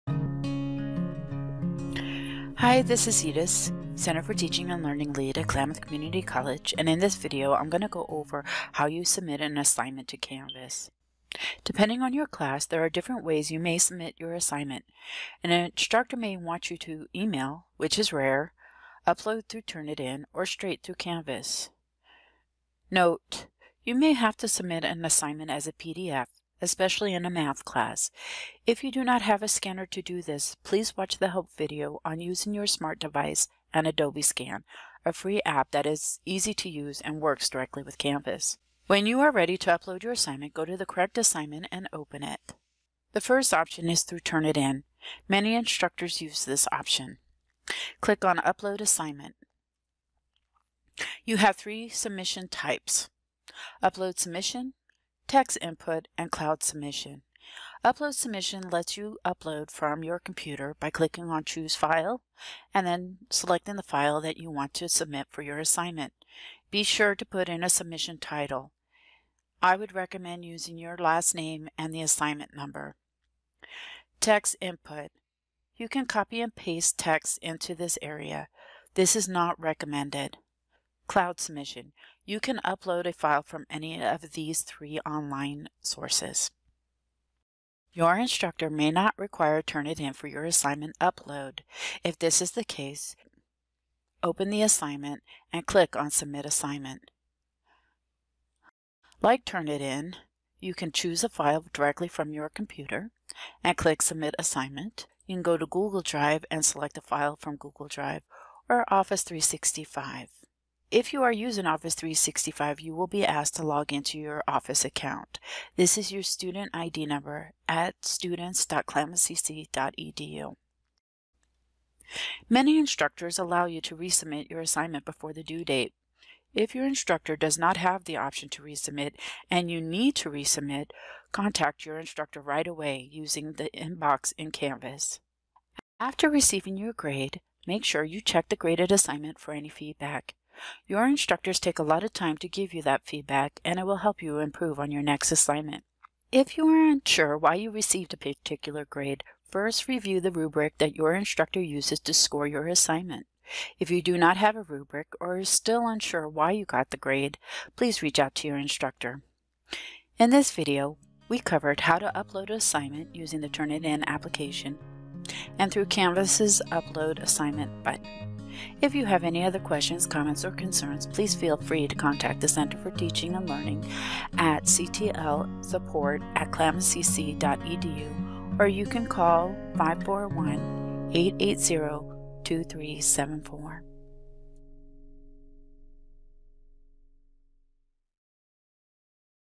Student Resource Video - Uploading Assignments in Canvas
How to upload assignment in Canvas using TurnItIn or the Canvas Submit button.